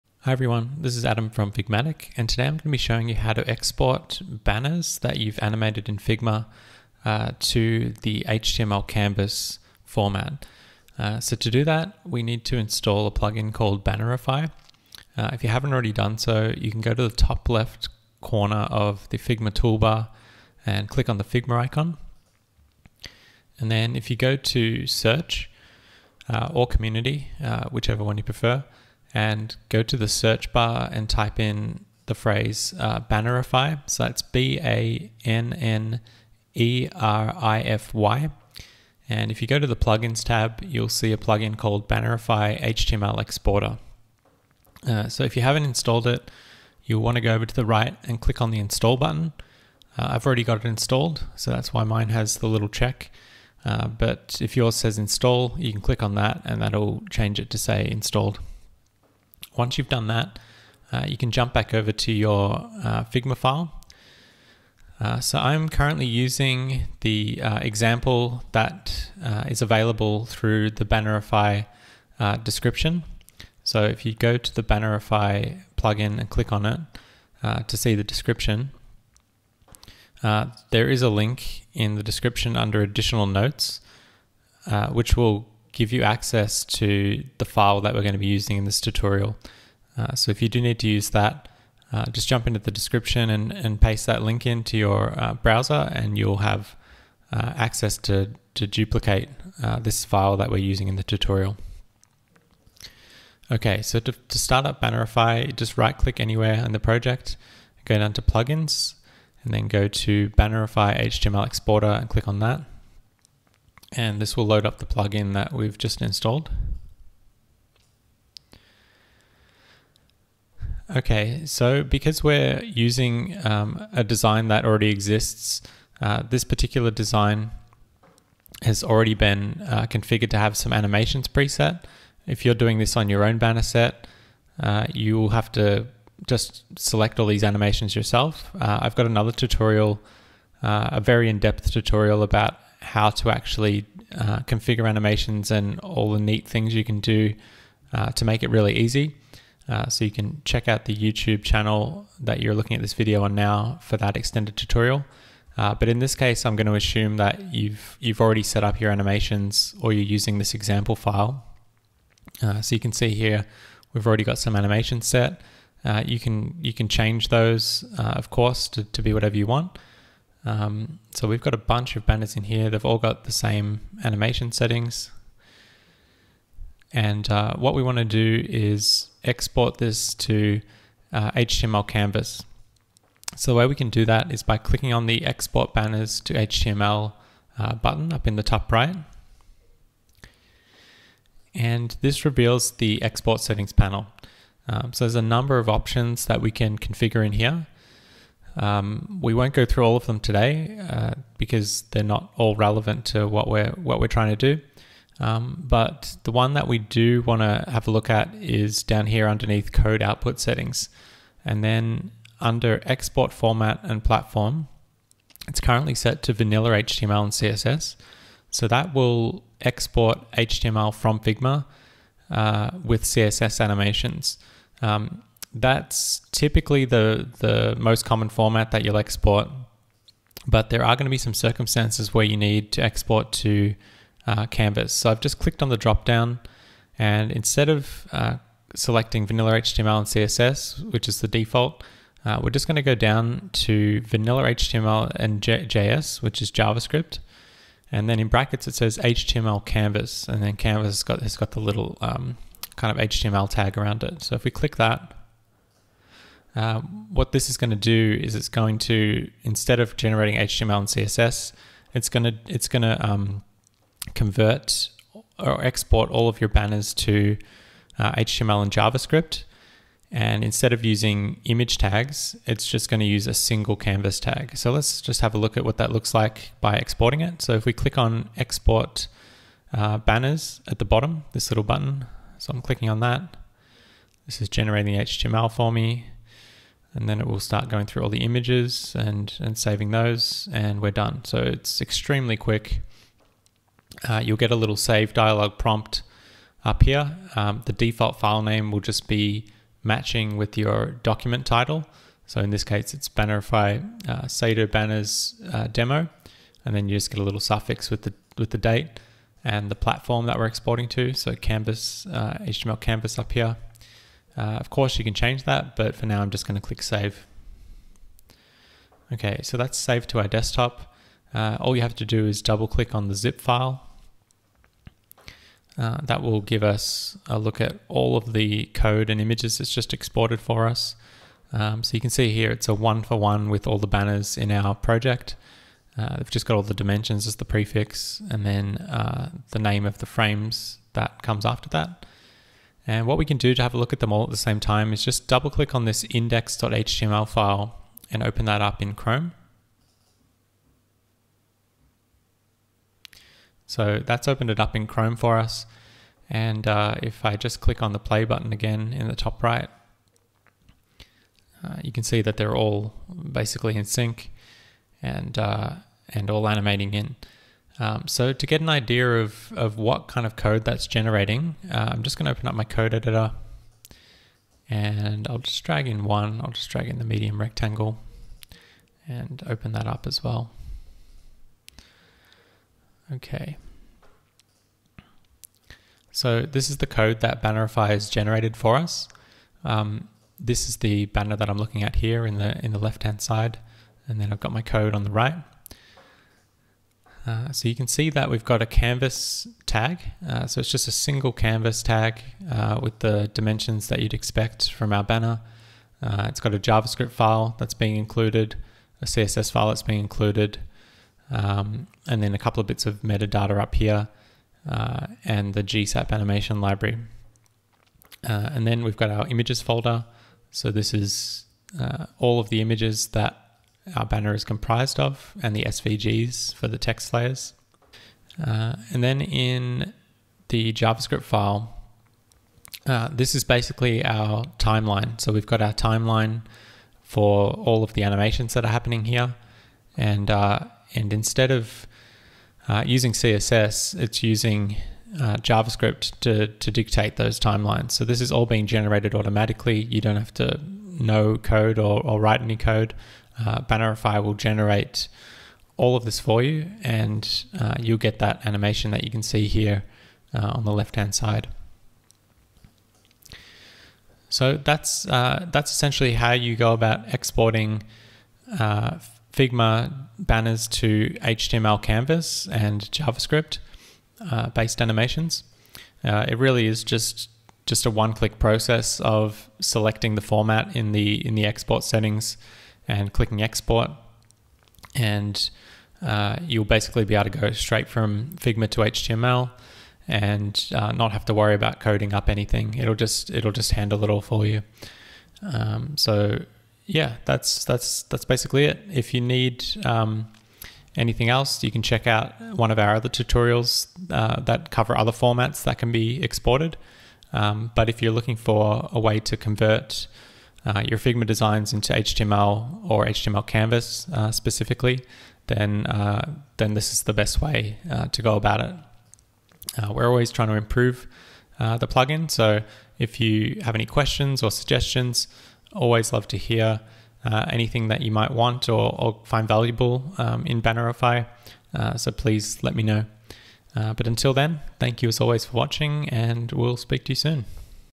Figma Tutorial: Export Figma to HTML Canvas
This video tutorial is a complete step-by-step guide showing you how to export production ready animated banners from Figma to HTML Canvas in seconds using the Bannerify plugin